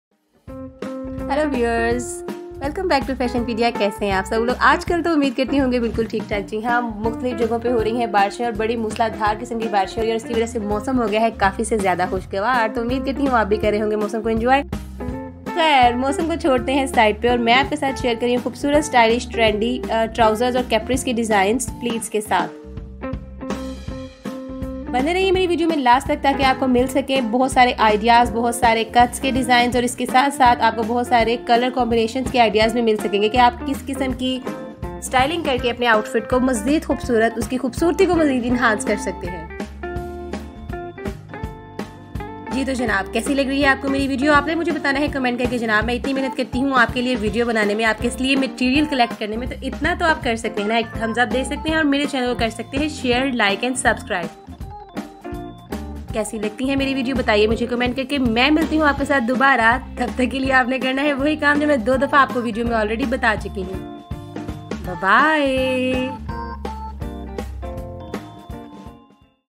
Summer Trouser Designs Ideas | Trouser Designs with Pleats | Fashion Pedia
Watch video on Summer Trouser Designs Ideas | Pleats Trouser Designs & get some inspiration.

FashionPedia - Ideas for Looking Good!